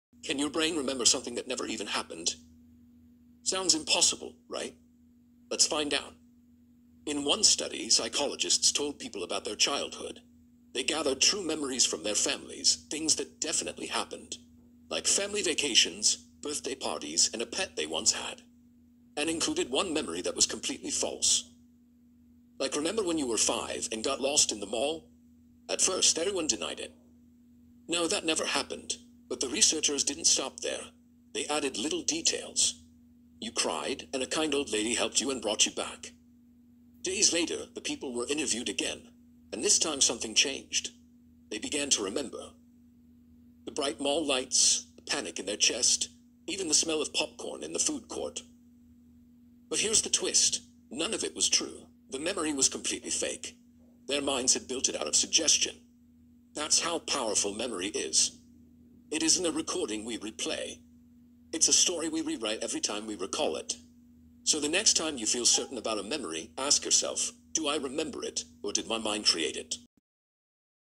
Your Brain's Biggest Lie?FalseMemory
Prepare to question everything you thought you knew about your past! 🧠 This short explores the fascinating "Lost in the Mall" study, where researchers successfully implanted vivid, yet completely false, memories into participants through suggestion and misinformation. Discover why your memory isn't a perfect recording, but a story constantly being rewritten. It's truly mind-blowing! What memories do you trust?